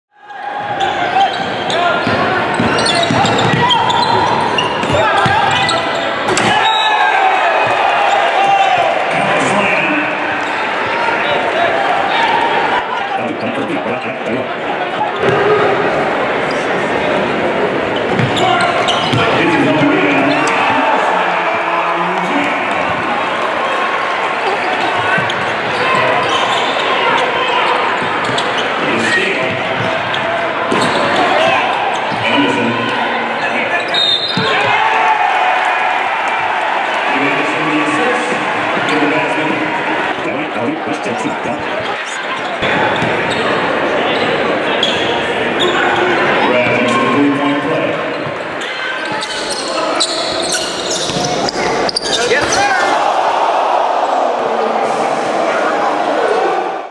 Ivan Rabb SLAMS Back To Back DUNKS!!! 7 Points Within 10 Game Seconds!!! NorCal Tip Off
Here's Ivan Rabb With 7 quick points for Bishop O'dowd
Special Thanks To: Gerry Freitas